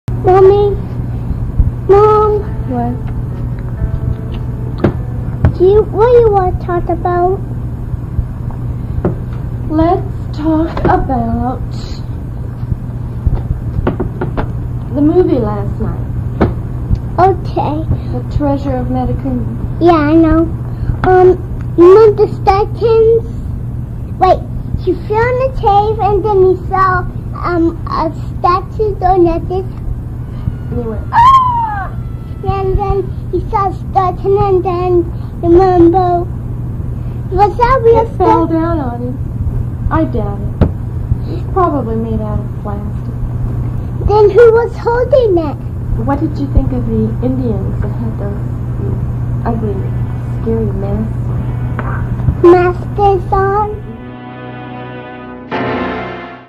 Audio from a 1976 audio cassette.
My mom and I discussing the movie we had watched the night before